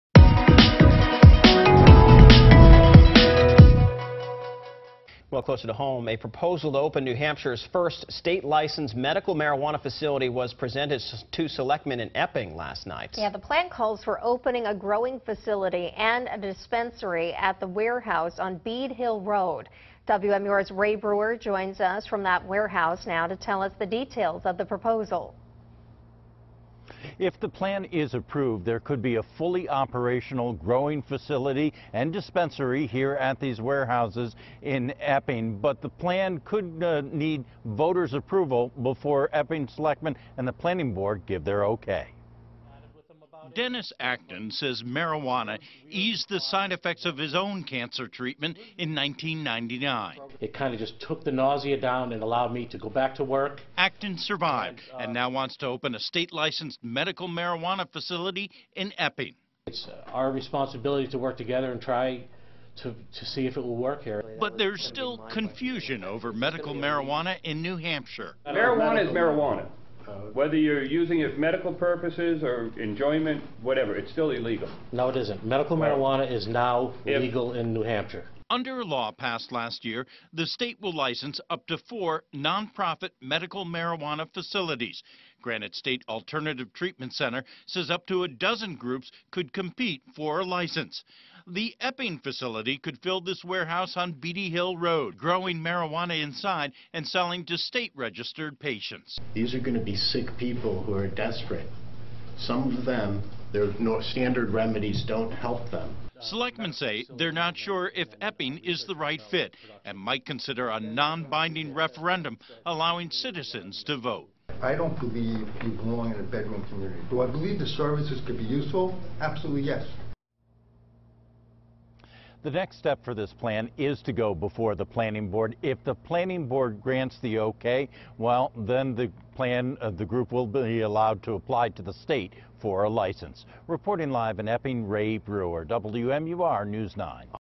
Epping considers state's first medical marijuana facility
A proposal for what could be New Hampshire’s first state-licensed medical marijuana facility was made to the Epping Board of Selectmen last night.